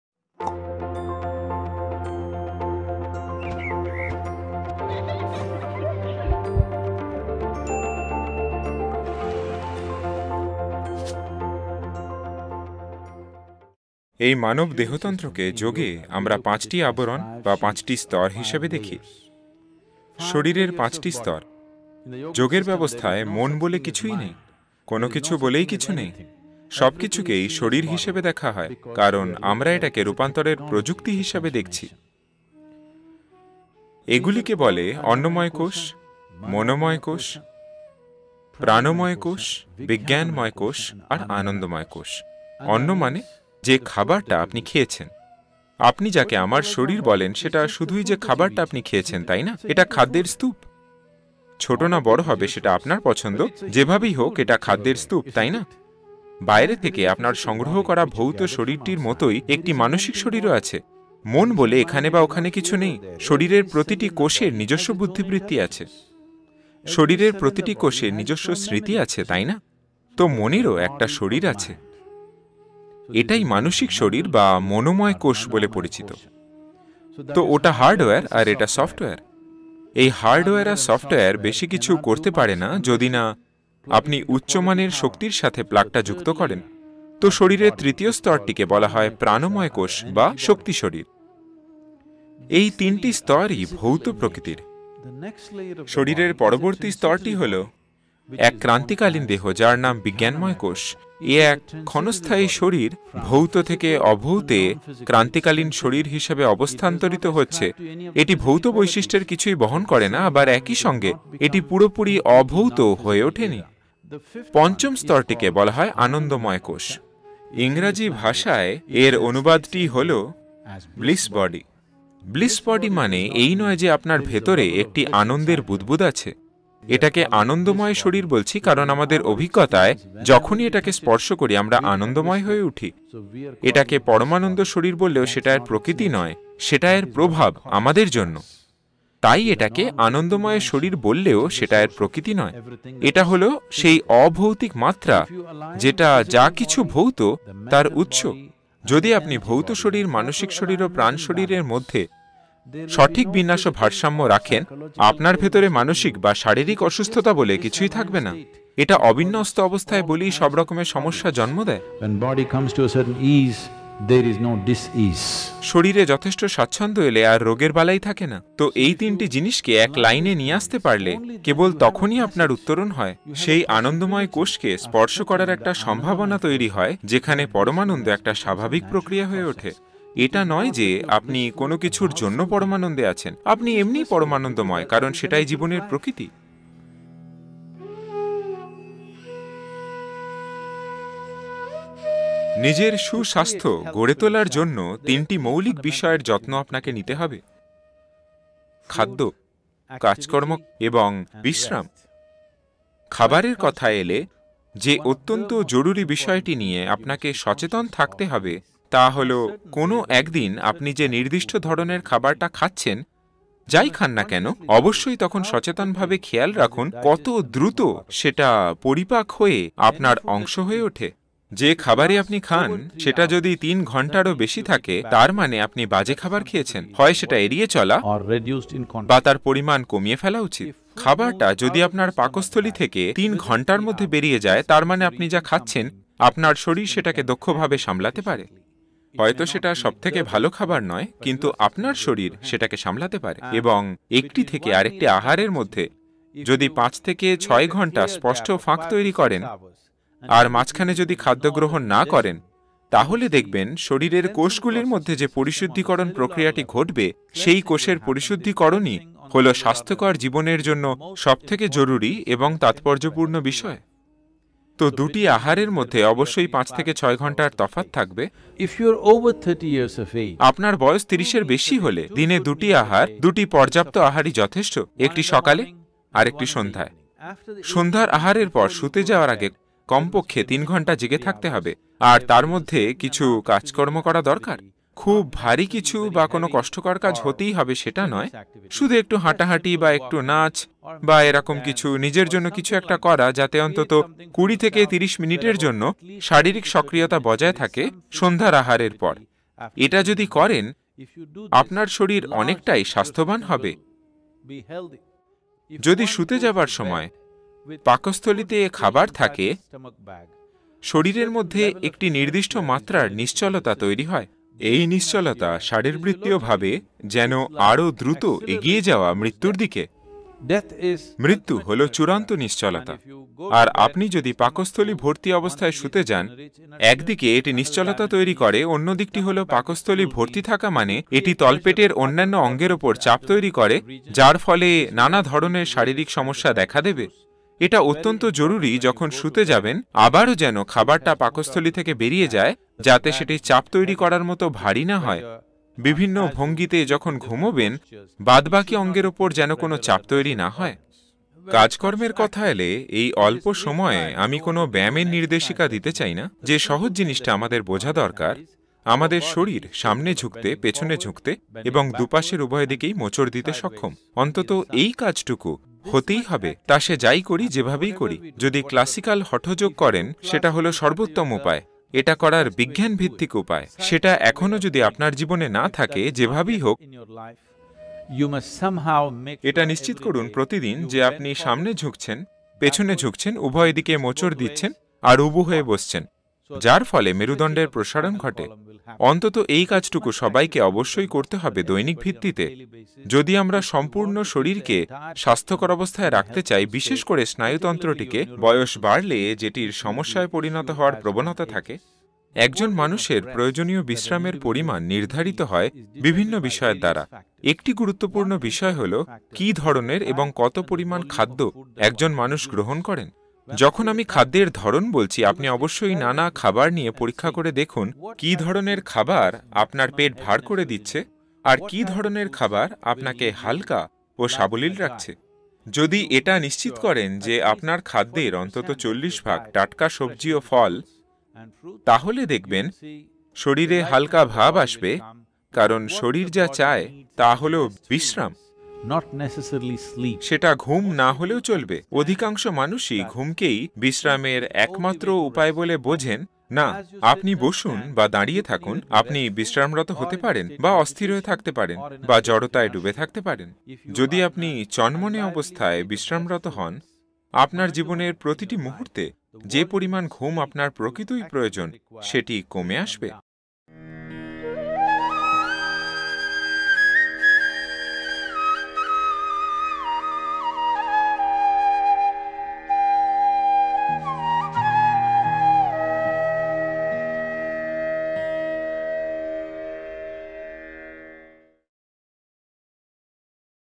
৫ মিনিট দিন এই শরীরের যত্ন নিতে | 5 Minutes For Health
এই ভিডিওটিতে সদগুরু আমাদের স্বাস্থ্যের জন্য ৫ মিনিটের অনুশীলনের বিষয়ে যোগের এক অনন্য দৃষ্টিভঙ্গি প্রদান করছেন। সাধারণ কয়েকটা মৌলিক বিষয়ে মনোযোগ দিয়ে আপনি নিজের সুস্বাস্থ্য নিশ্চিত করতে পারেন।